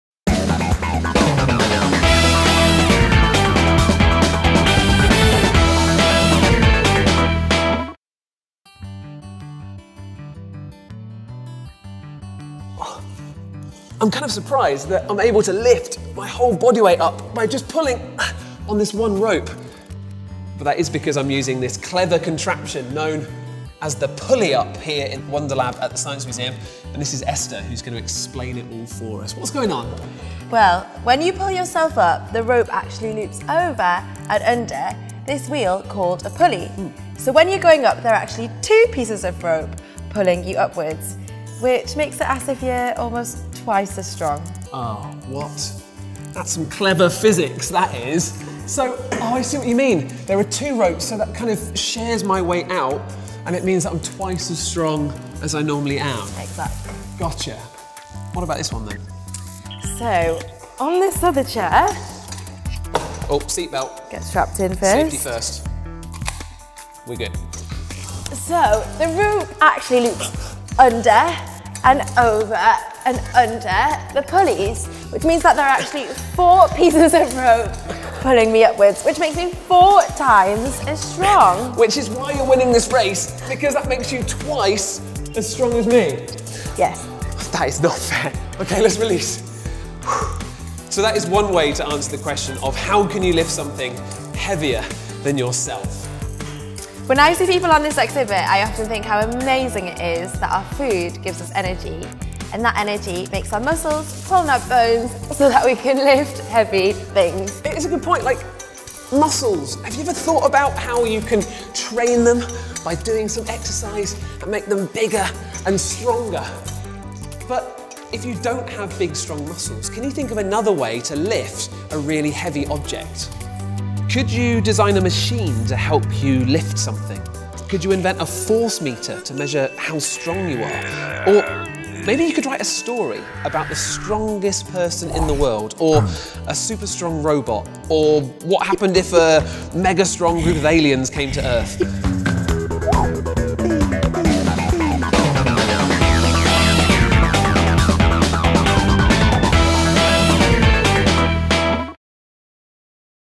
Can you lift something heavier than yourself? 💪🏋🏿 | STEM for kids
Have you ever tried to lift something that was heavier than yourself? Do you think this is even possible?

This video will help your students explore pulleys and make creative connections between forces, literacy, PE, history, art and design.

Filmed at Wonderlab: The Equinor Gallery at the Science Museum, London.

Want to explore our educational activities and interactive games? Browse our full list of learning resources